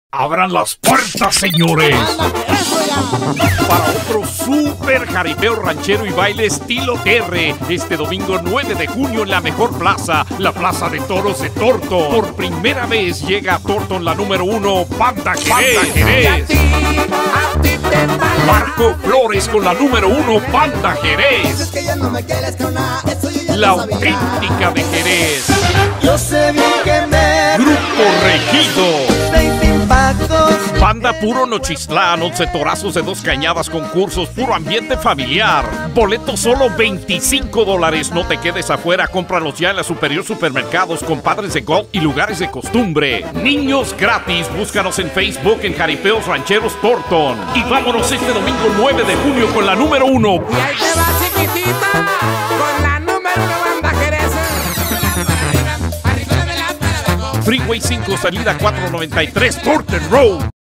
Domingo 9 de Junio Plaza de Toros Thornton La #1 Banda Jerez
Domingo 9 de Junio Plaza de Toros de Thornton Jaripeo Ranchero y Baile
La #1 Banda Jerez, La Autentica De Jerez,Grupo Regido
11 Toros Pura Lumbre,$25 preventa y niños Gratis 100% Familiar